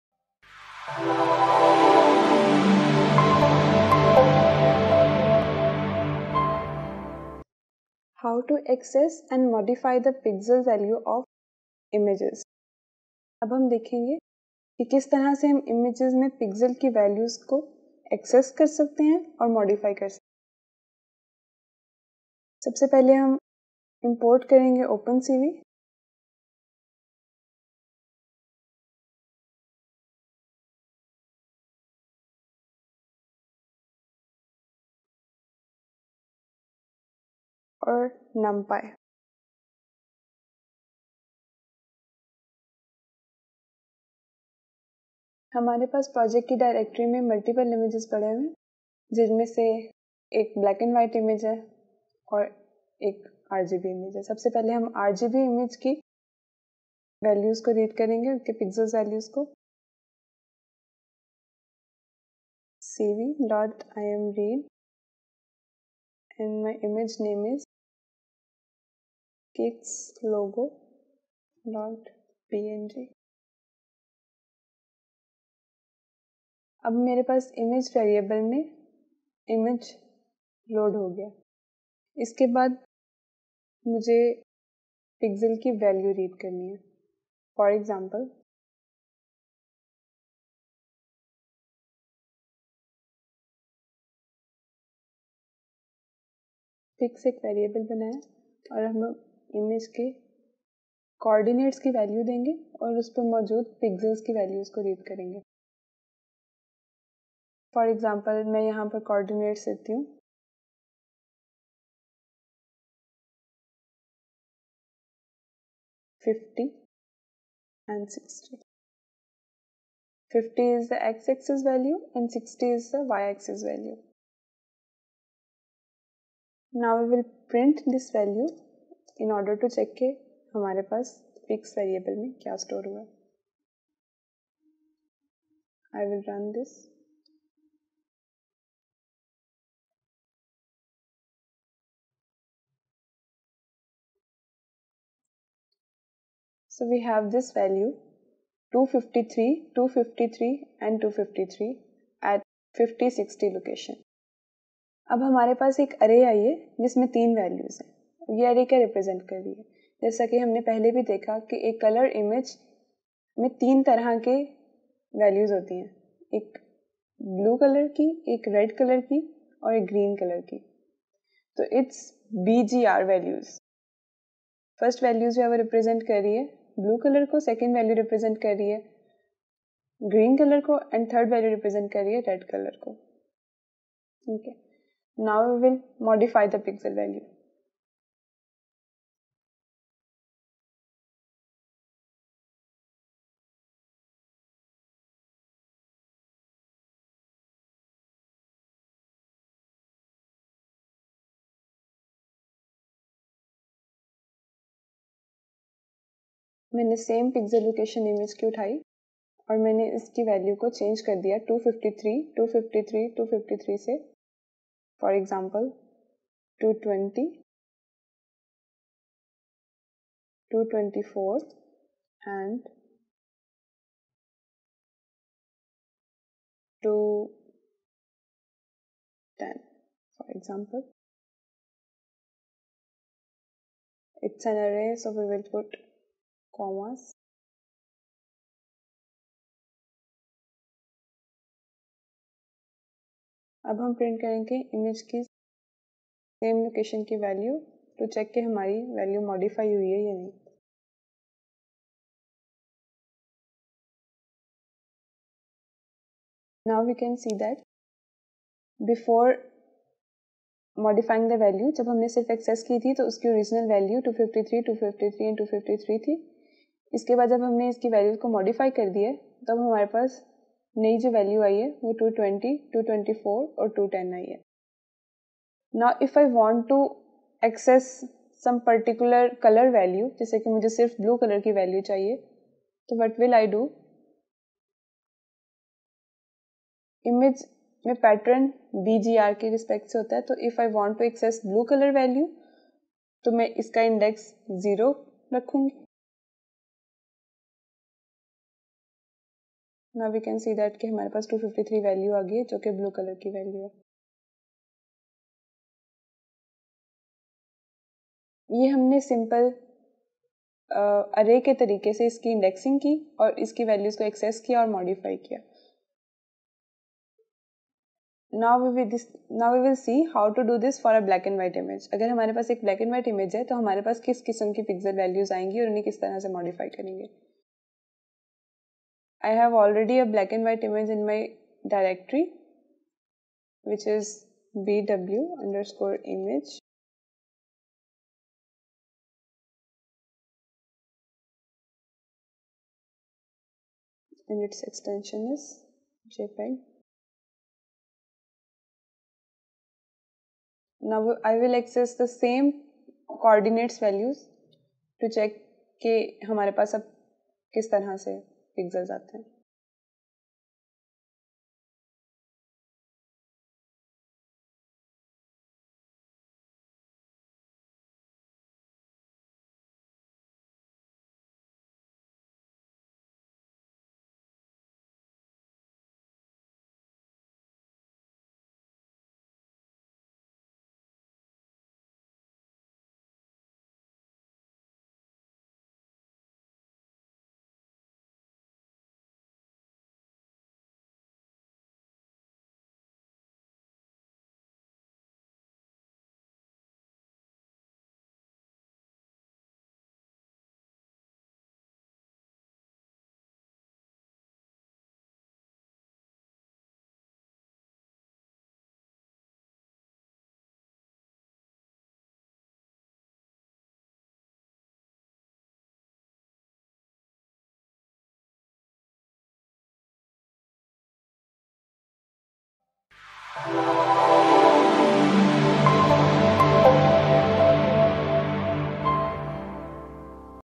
OpenCV Tutorials For Beginners (09) Access and Modify Pixel Values Using OpenCV || Urdu || Hindi
Welcome to OpenCV tutorials for beginners, in this video we will learn how to access and modify pixel values using OpenCV in python.
OpenCV tutorials for students and beginners by Saira Jabeen